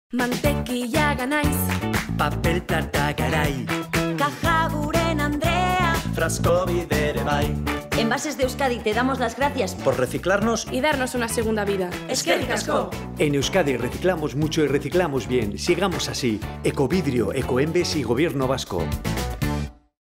🔵🟡🟢♻️ EKOADRILA - En Euskadi reciclamos mucho y reciclamos bien, ¡sigamos así! [CAS]
🔵🟡🟢♻️ Envases de Euskadi te damos las gracias por reciclarnos y darnos una segunda vida. Eskerrik asko! En Euskadi reciclamos mucho y reciclamos bien, ¡sigamos así! 🙌🏻